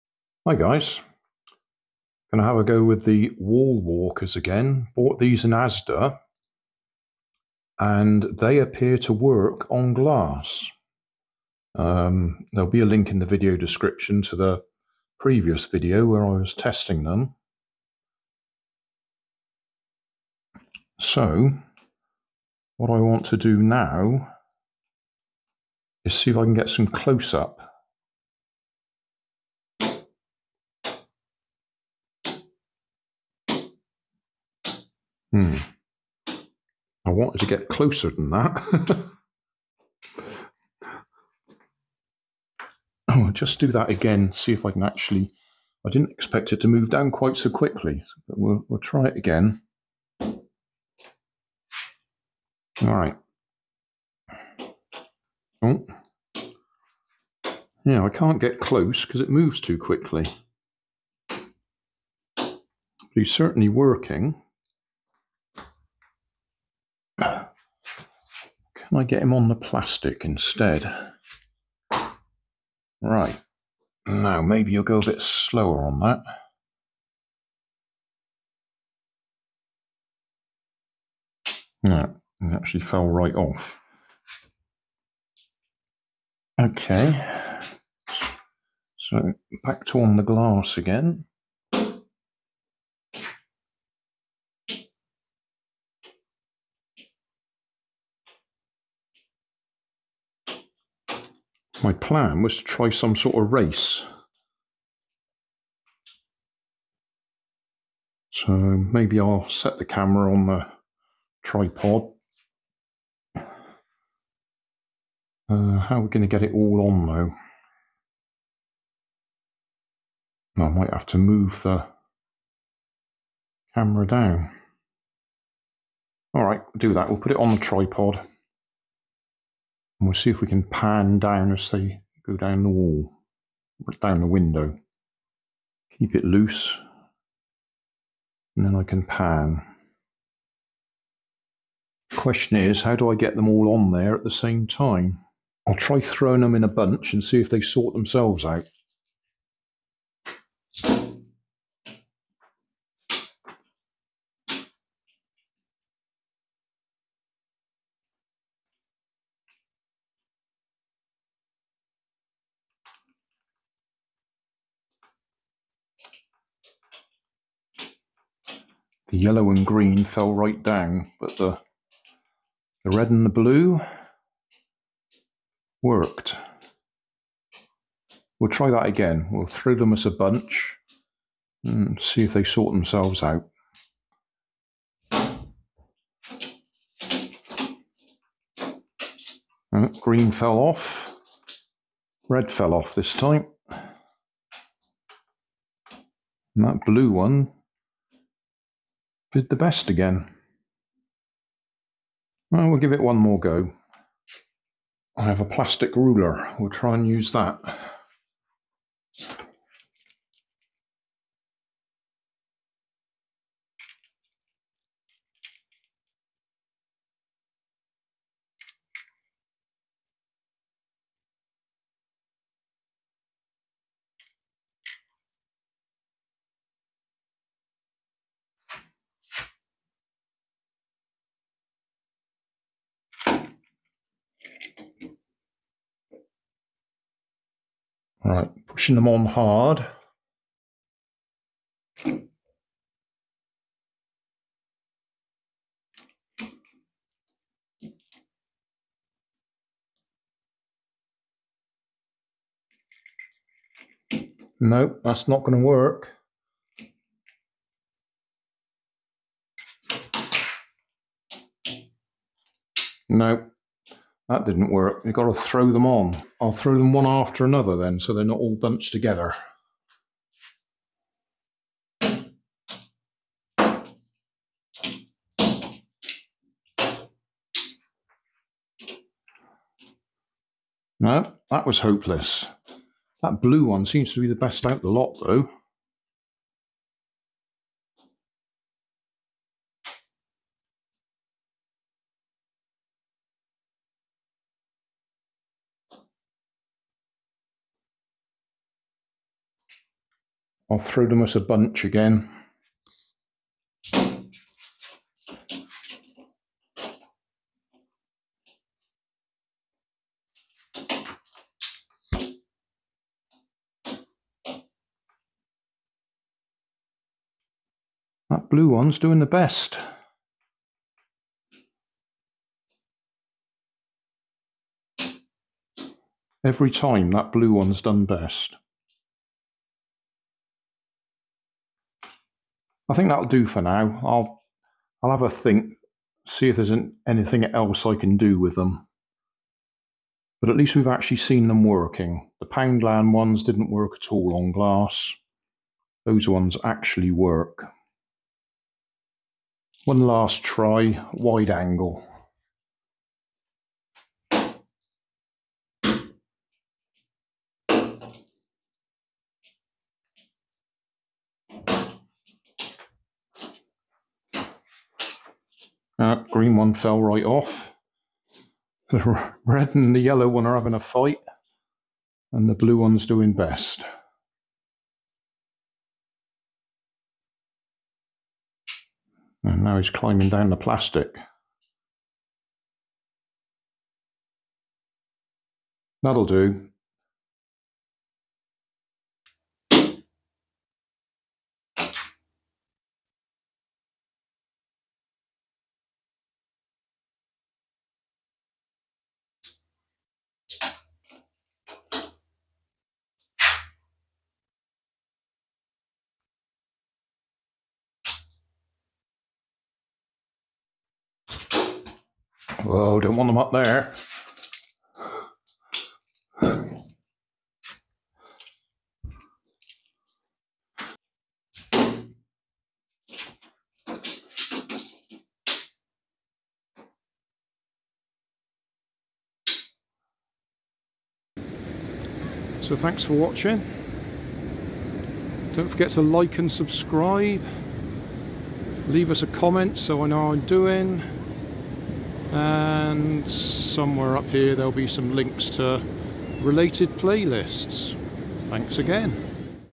Wall Walkers from ASDA 20240108 EXTRA
Wall Walkers from ASDA 20240108 EXTRA. After the trouble I had with the Poundland set staining the white paint on the wall, you might think I would have avoided this. I noticed the instructions on this set clearly warned you NOT to use them on internal walls due to the possibility of marks and staining.